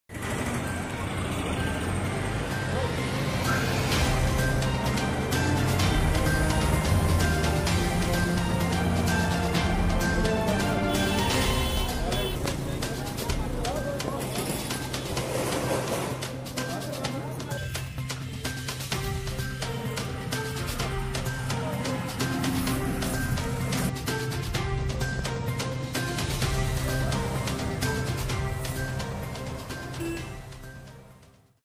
*Downtown parts, Masuma shut against Awantipora teacher's death in custody* Downtown areas and als
*Downtown parts, Masuma shut against Awantipora teacher's death in custody*

 Downtown areas and also the Maisuma areas observed spontaneous shutdown against the death of a school teacher from Awantipora in police custody.

Witnesses told  that the shopkeepers downed their shutters in downtown areas as the announcement was made in  Jamia Masjid asking people to observe shutdown.

Similarly shutdown was also observed in Maisuma as well.